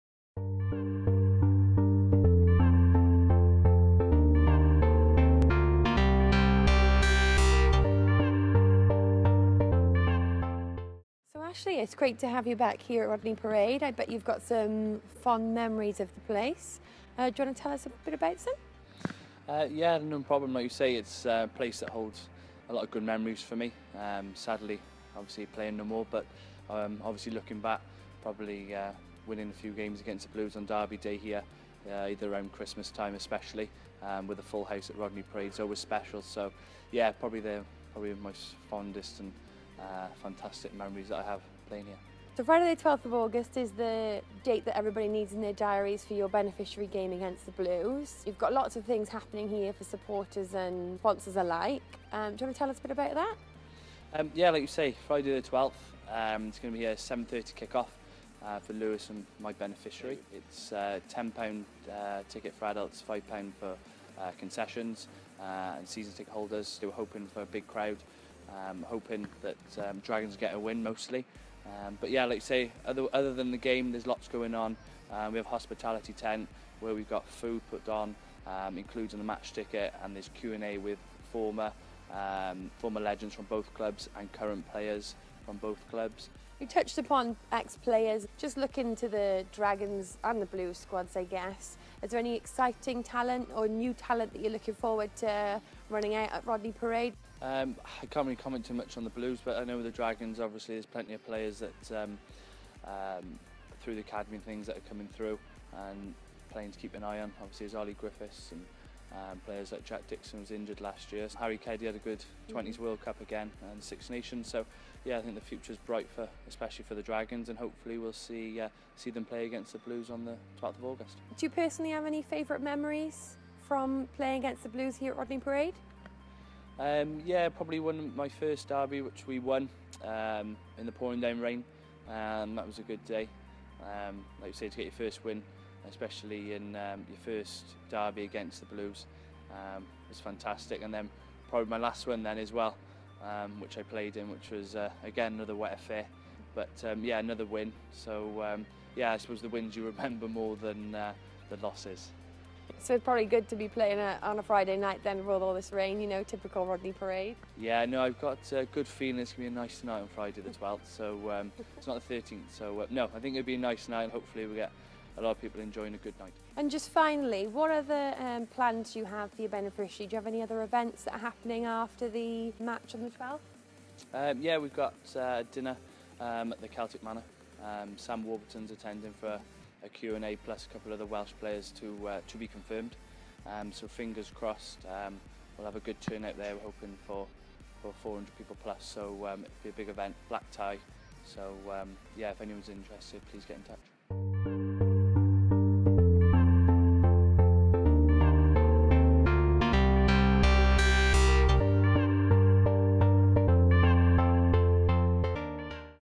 Ashley Smith - Beneficiary Match Preview
Dragons TV catches up with Ashley Smith to look ahead to his and Lewis Evan's Beneficiary Match which sees the Dragons face Cardiff Blues on Friday 12th August (7:30pm).

Tickets for the match are available now priced at £10 for Adults, £5 for Dragons Season and General Members (discounted rate) and £5 for Concessions/Under 16’s.